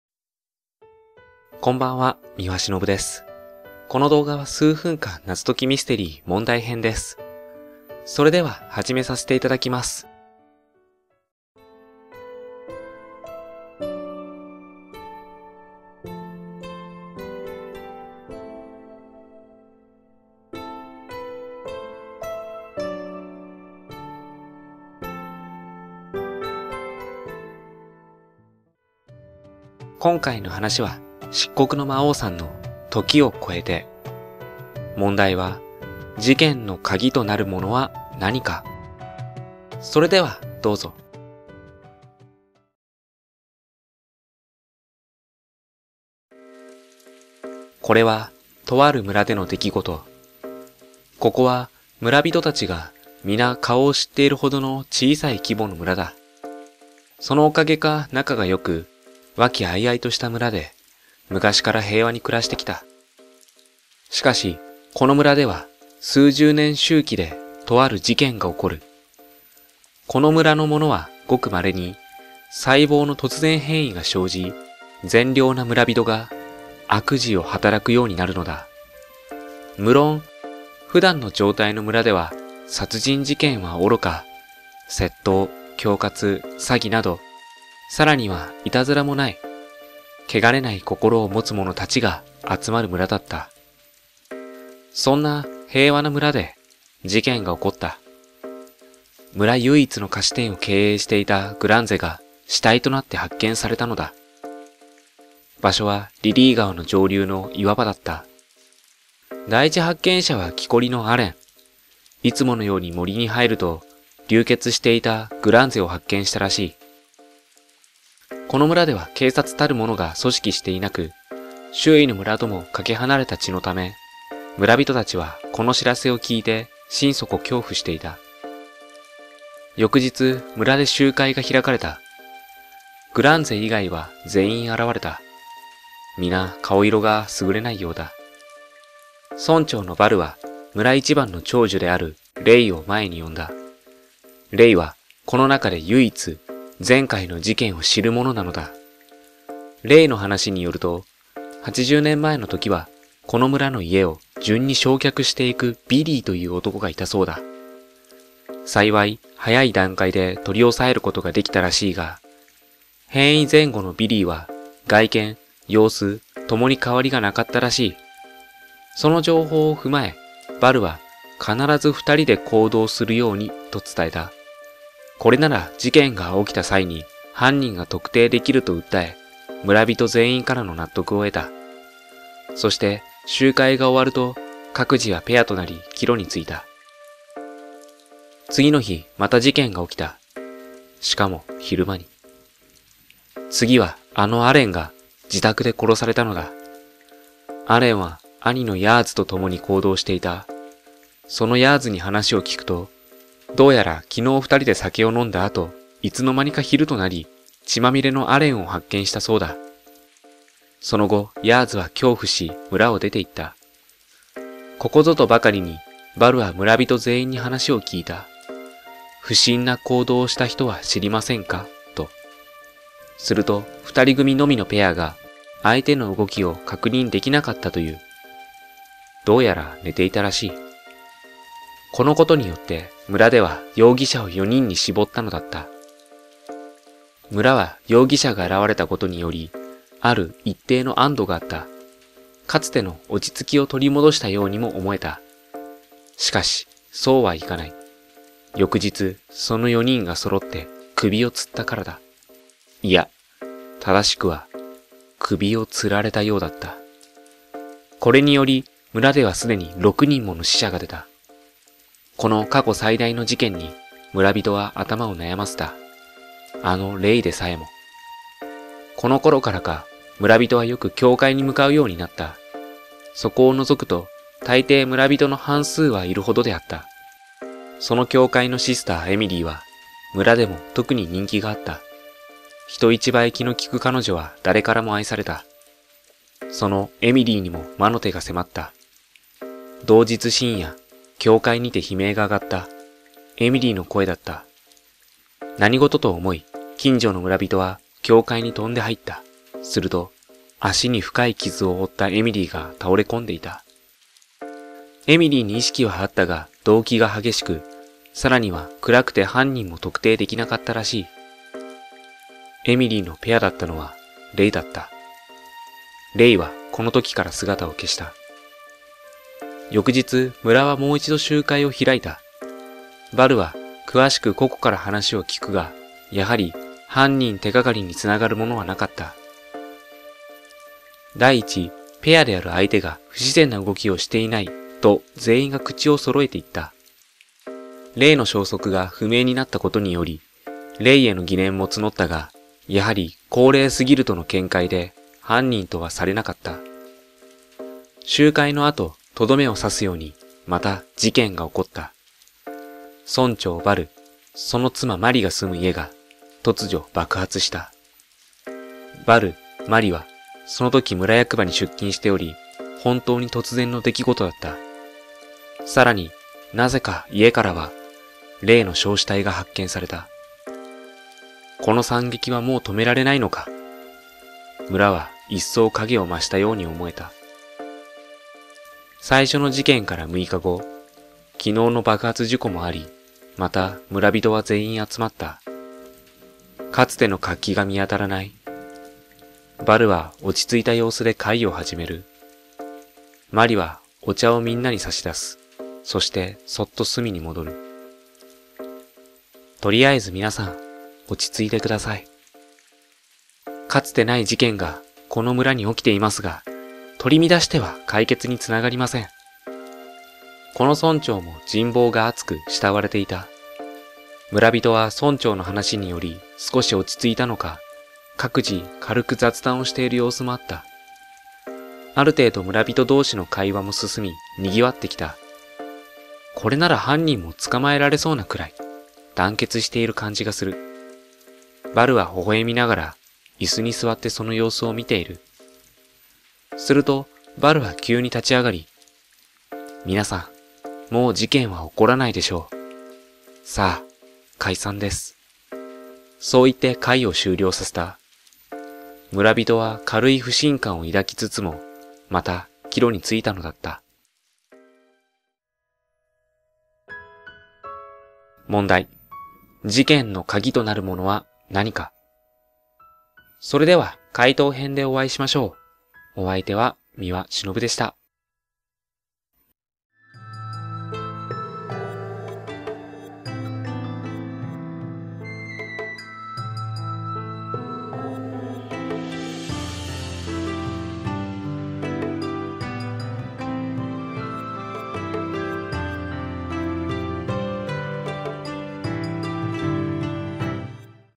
【問題編】数分間謎解き推理本格ミステリー「時を超えて」
解答編はこちら（翌日夜10時更新予定）
　ー『推理は、答えを覗き見て辿り着く場所ではない』

皆様からのご意見・ご感想・ミステリー問題など募集しております。